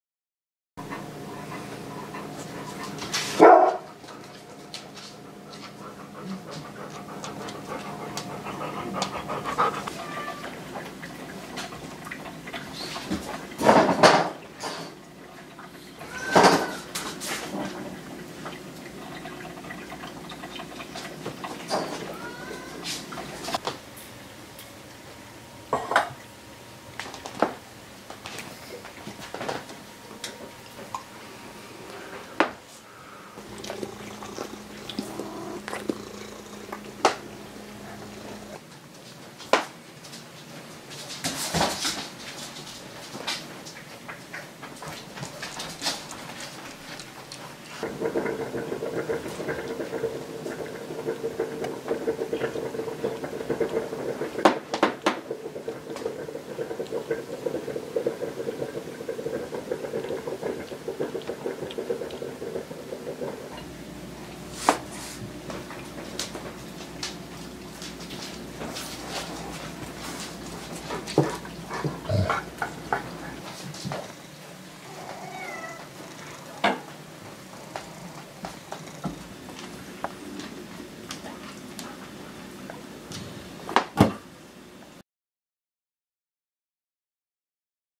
Siberian Husky and Cat Love Compilation
Siberian Husky and Cat love compilation!  Assorted clips on how good our Siberian huskies and malamute cohabitate with our cats!  We have a house full of amazing animals!